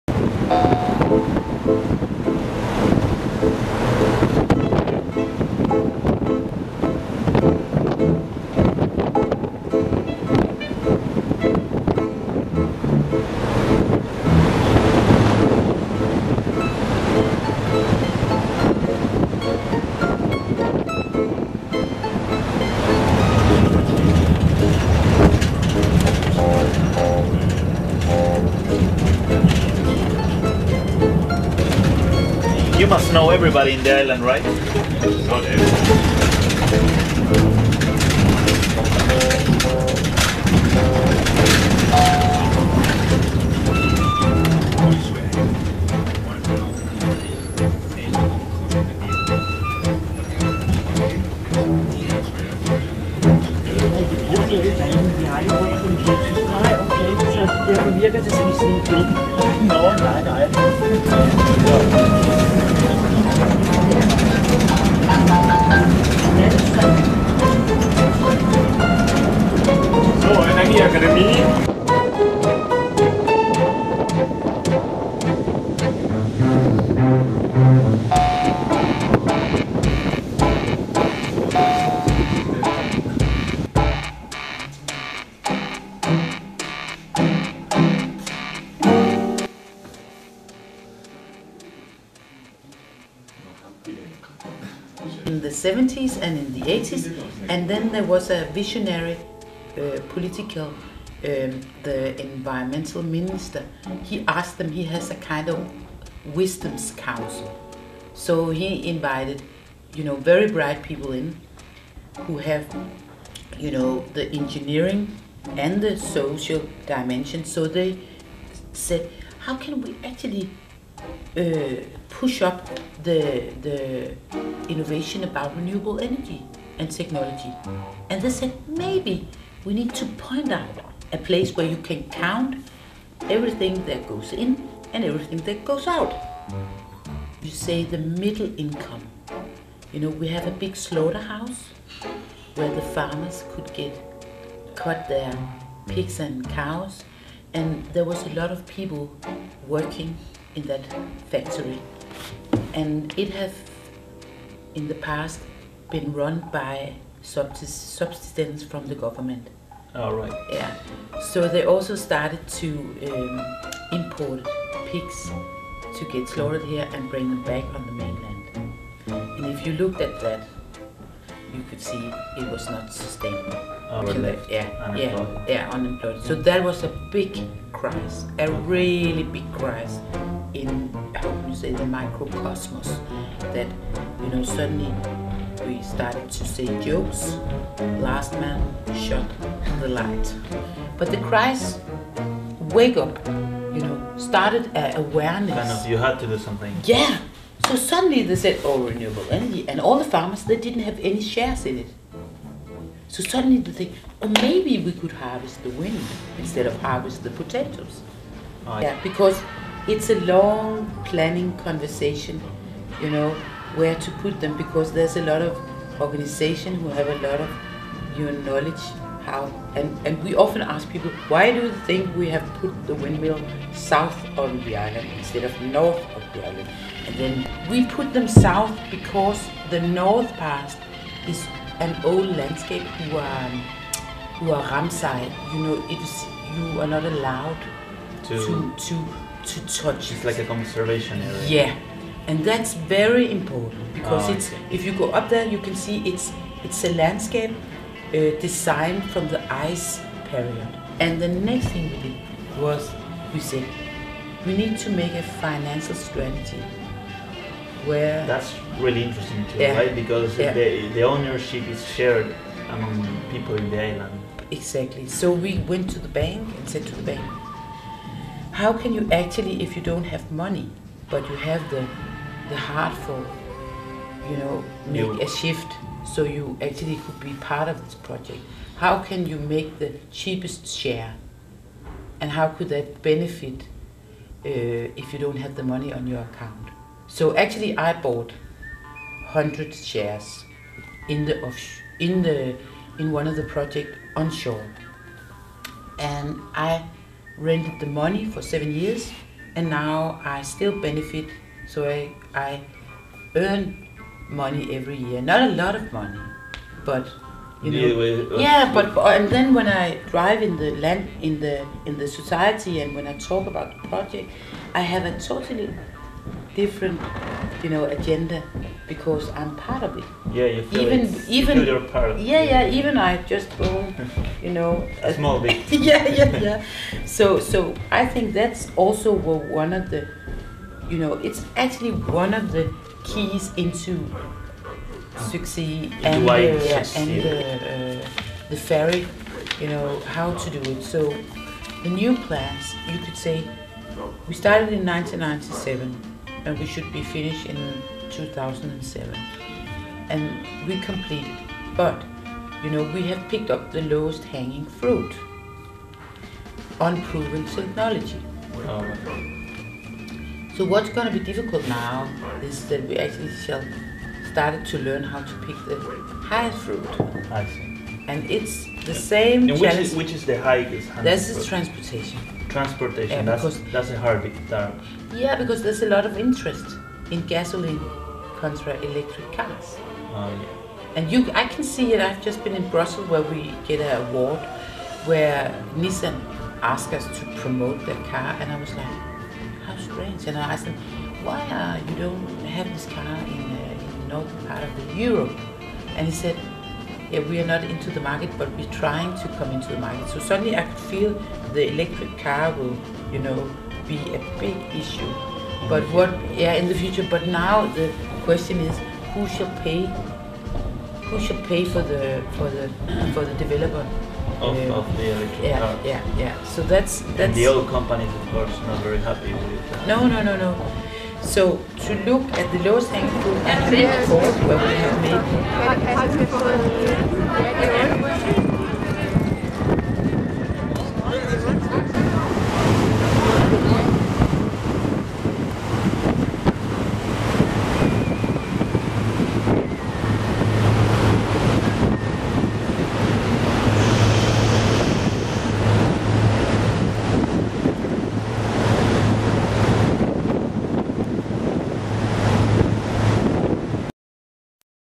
Visit to Samso Energy Academie
Summer 2012 visit to the Samso Energy Academy in Denmark.